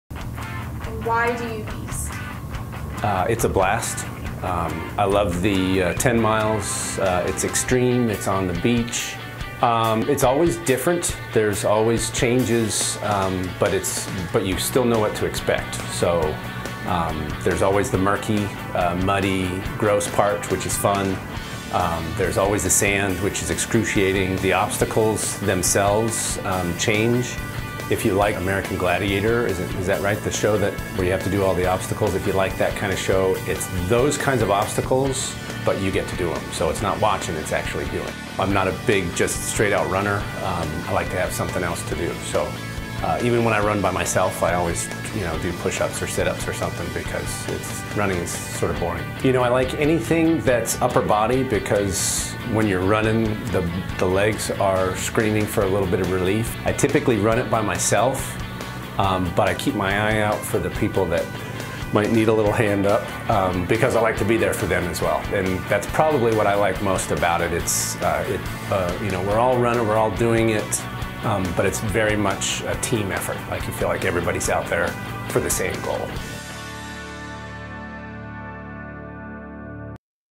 Why I Beast: Rob Green
Everyone Beasts for different reasons, whether it's for a loved one, for the challenge or for the comradery on the course. See what drives Beast Ambassador Rob Green to come back, year after year.

The 7th annual Barber Beast on the Bay is set for Saturday, September 7, 2019, at Presque Isle State Park in Erie, PA.  Participants will take on 10 miles of sand, water, and trails – it’s the nature of the Beast!

A one-mile adapted course will also be offered for those ages 12+ with physical or intellectual disabilities at Waldameer, a theme park in Erie, PA next to Presque Isle State Park.